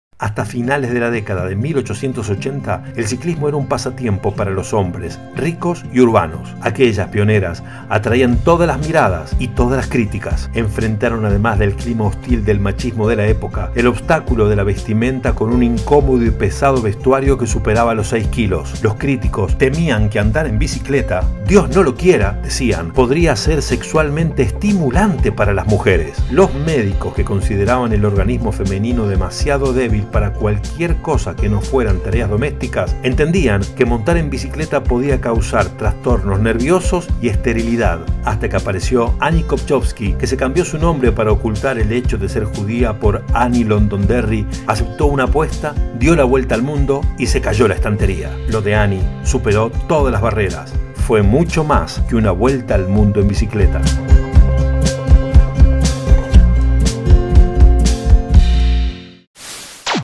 CUANDO LAS MUJERES DECIDIERON TOMAR LAS BICICLETAS
Aquí la historia de cuando las mujeres decidieron tomar las bicicletas. Otra conquista.